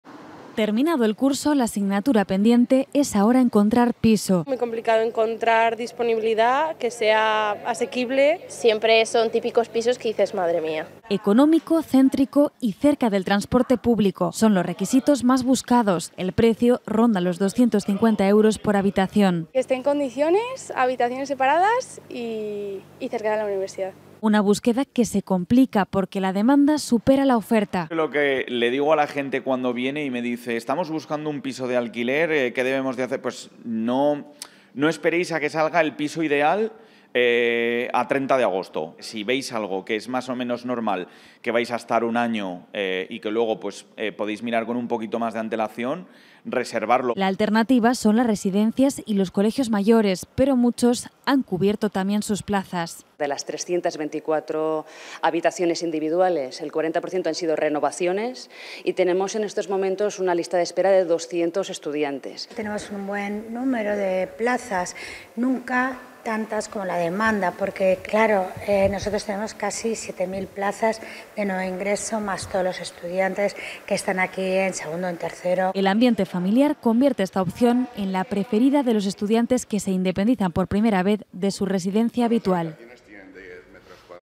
La aventura de buscar alojamiento de estudiantes
Mucha demanda y poca oferta: así es la búsqueda de vivienda de los estudiantes en Aragón. La opción preferida es la de piso compartido...el precio por habitación ronda los 250 euros y muchos de estas viviendas están completamente desactualizadas...por eso muchos optan por residencia o colegios mayores.